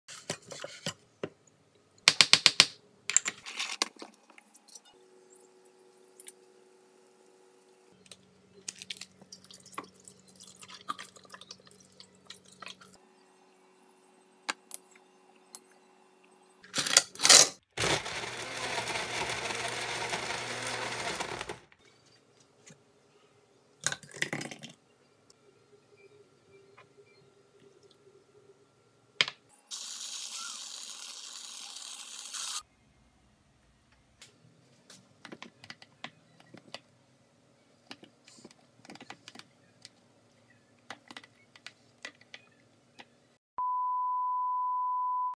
[프라푸치노] 홈카페/그린티 프라푸치노/샷 그린티 프라푸치노/일리커피/모카포트
더워요. 너무 더워요.😭 더울때는 역시 프라푸치노...:))
오늘은 샷 그린티 프라푸치노를 만들었습니다.
시원하니 좋네요.
그런데 역시 프라푸치노는 우유로 만들어야 더 맛있다는 것...😅
오늘도 즐거운 하루 보내세요...:))

그린티 파우더(쟈뎅)
아몬드 브리즈(매일유업)
에스프레소(일리 블랜드)
휘핑크림(메글레 스프레이 크림)
얼음(직접 얼림)
모카포트(비알레띠 2잔용)